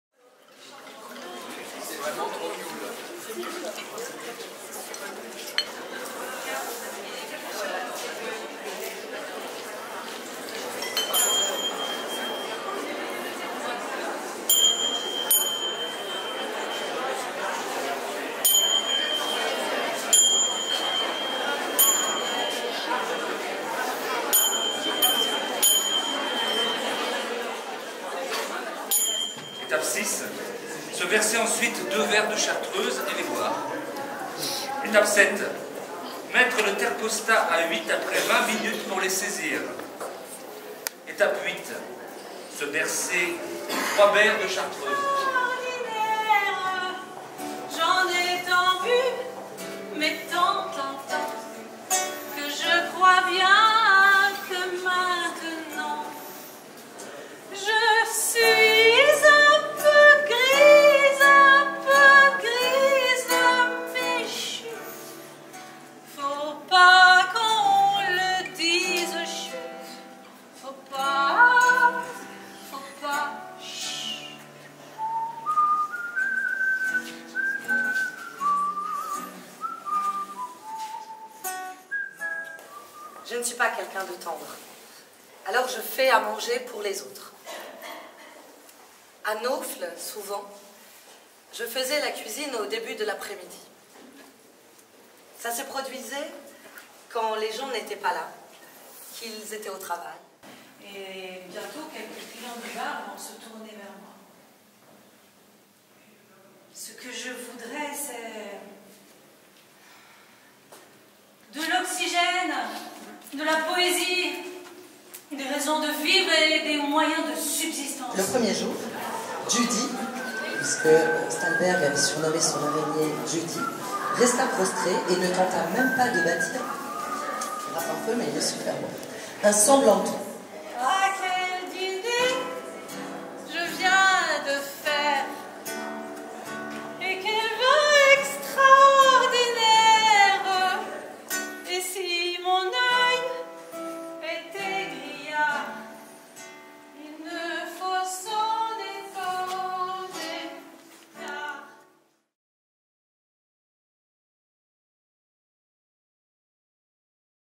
Apéro festif aux Caves de Chartreuse - Livres à vous 2018
Moment festif, théâtral et gastronomique aux Caves de la Chartreuse, pour clore les rdv artistiques des Autok'art du festival Livres à vous 2018 à Voiron et dans le Pays Voironnais.
Paroles et petits airs composés par la compagnie La peau de l’ours, avec Marijke Bedleem, Stéphane Daublain et Véronique Ferrachat.